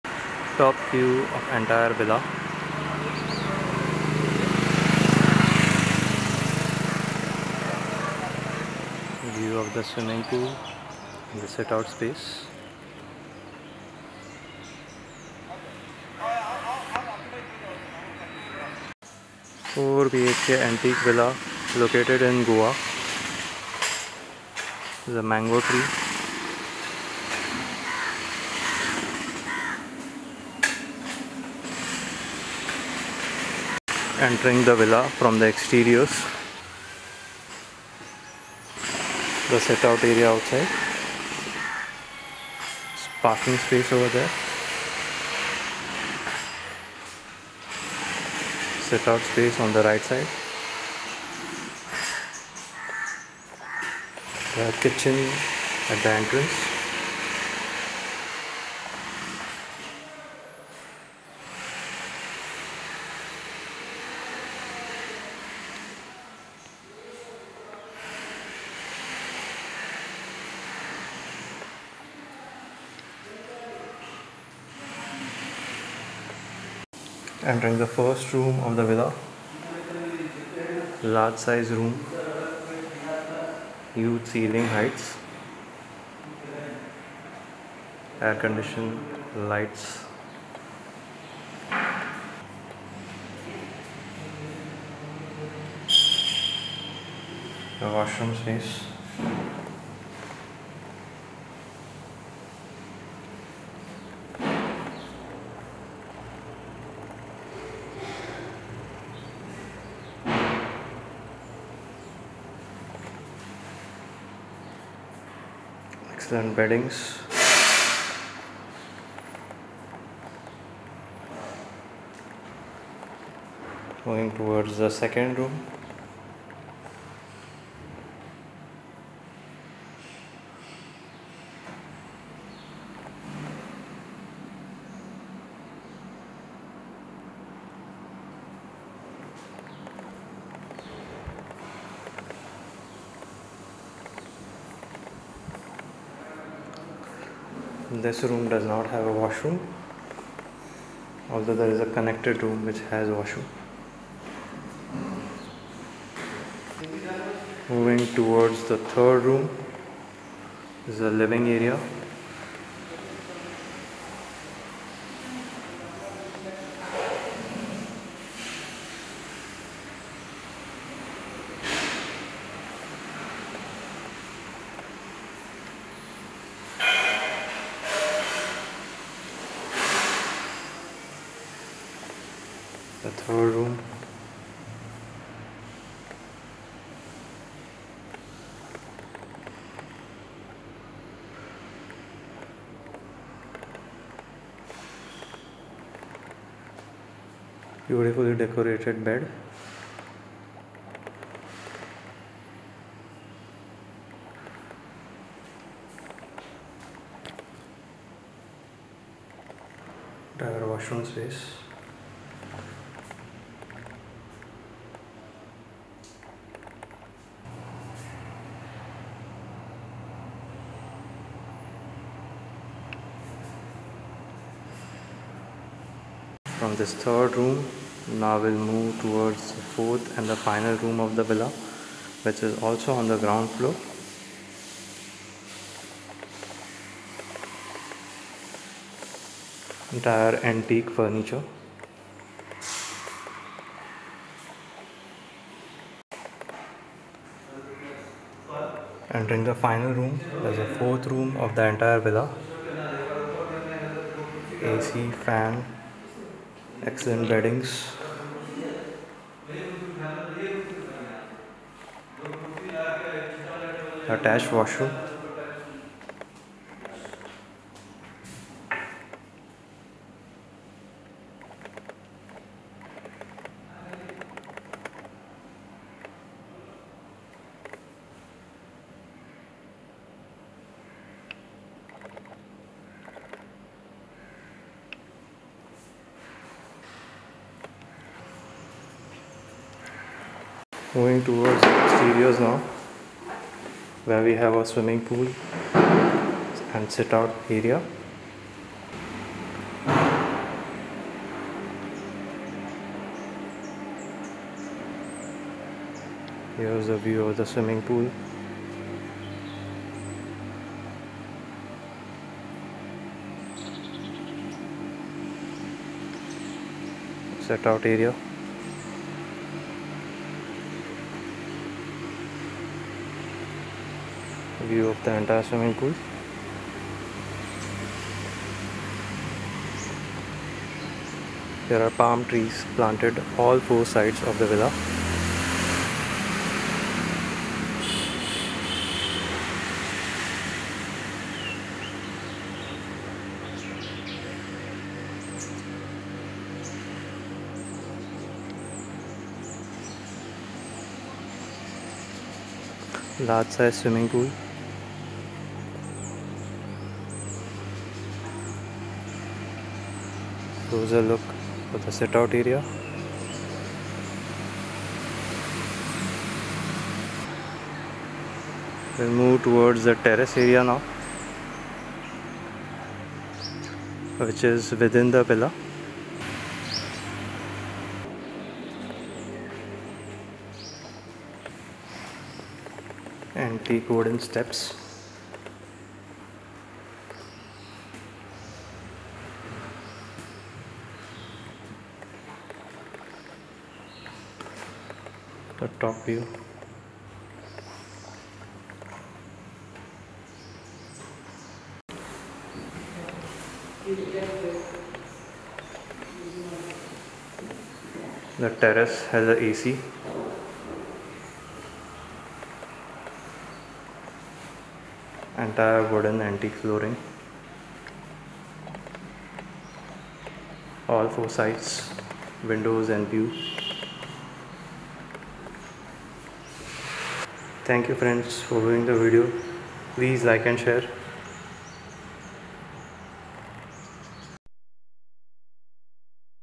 4bhk Antique Jungle Villa Goa Private Pool, 10000 Sq ft
Classical 4bhk AC Villa with Private Pool , Huge Living Room Space, Garden & Covered Terrace. Fully Equipped Kitchen, Caretaker, Parking Space. All 4 Rooms are on Ground Floor of the Villa.
It will help us to create more and better videos and keep improving always. Its a Holiday Home.
Approx Price Range : 15k to 30k for 8 guests